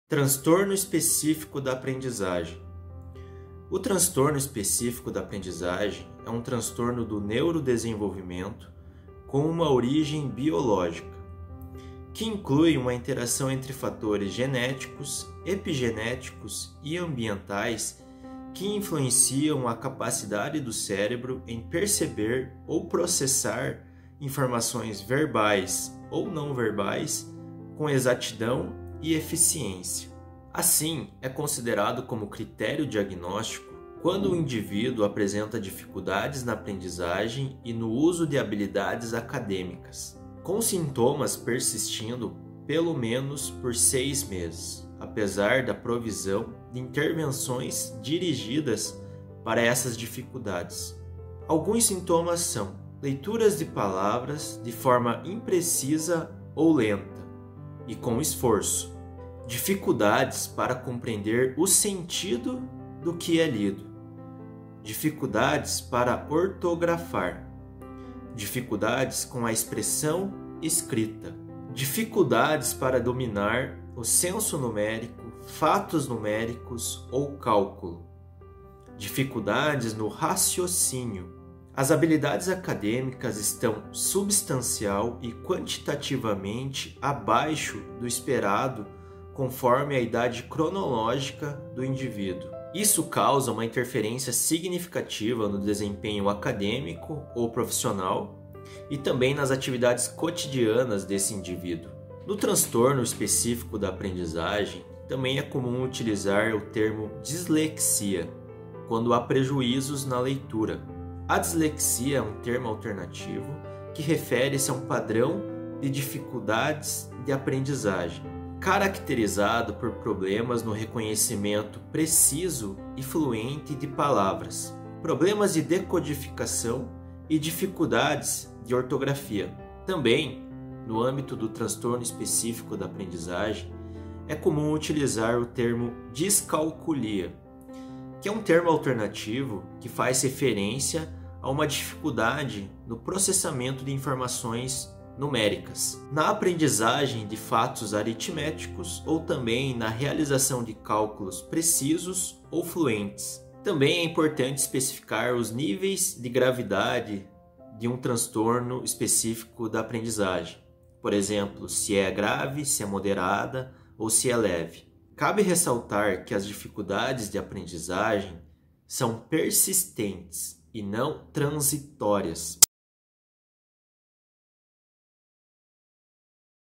Como identificar Transtorno Específico da Aprendizagem?
Características do transtorno específico da aprendizagem.